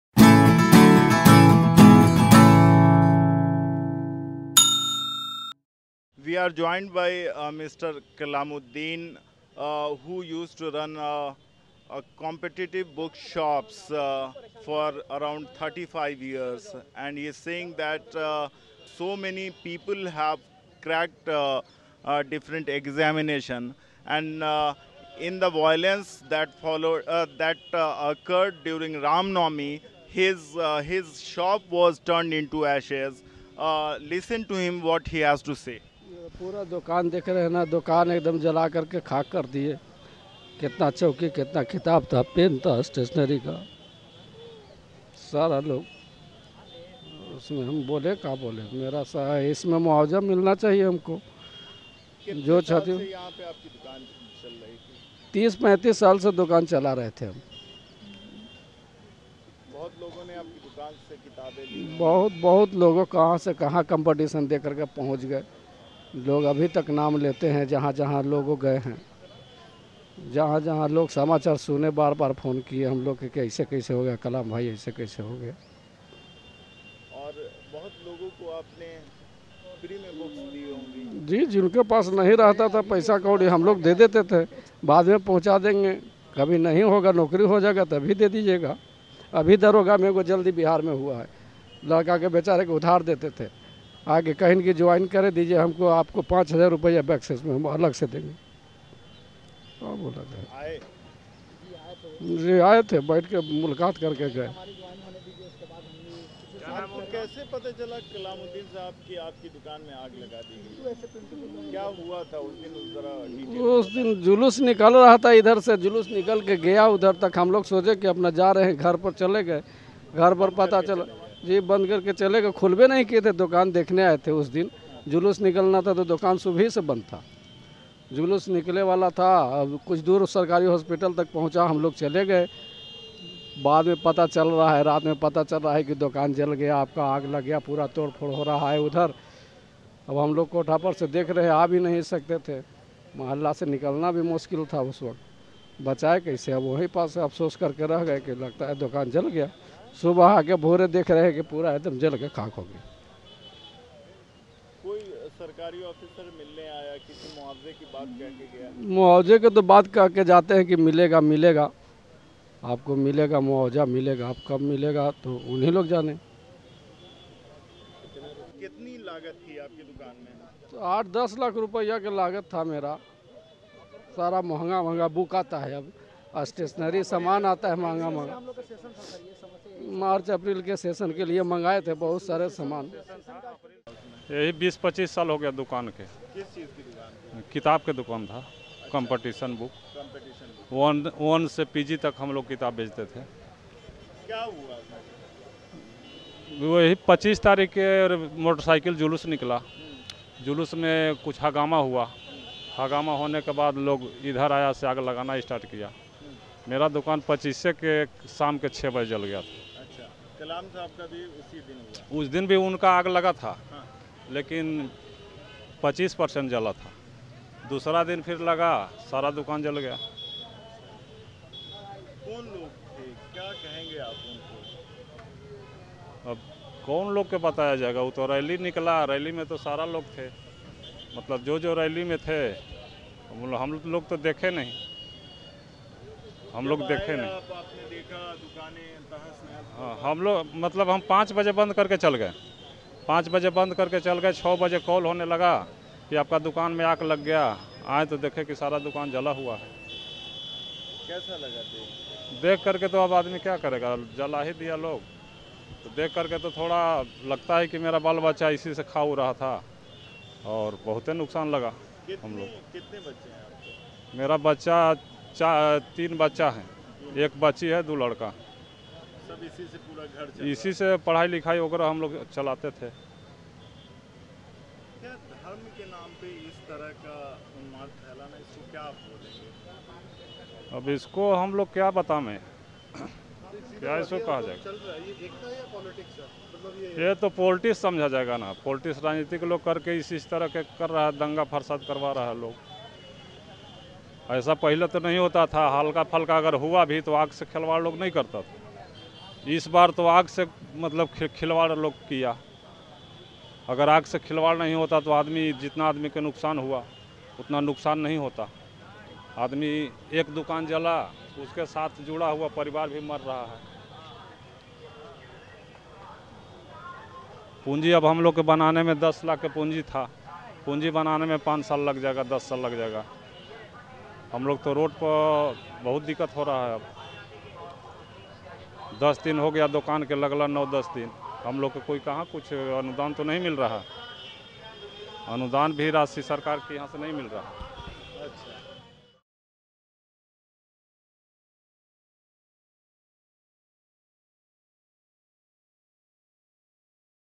Manufactured Riots in Bihar - Survivors and Perpetrators
The communal violence that took place in the nine districts of Bihar from March 25 to 28 was orchestrated and well-planned. The scale of direct communal instigation that played a major role in the violence that followed, has not been witnessed in the state of Bihar, in the recent times.

The riots have claimed one life, while around 65 people were injured, as the Rama Navami processions turned violent in these districts. The communal tension and fear is palpable, even in the areas that previously had seen people of all the communities coexisting peacefully.

To take stock of the situation, the NewsClick team travelled to Bihar's Aurangabad district and spoke to the families and people, who have survived the riots.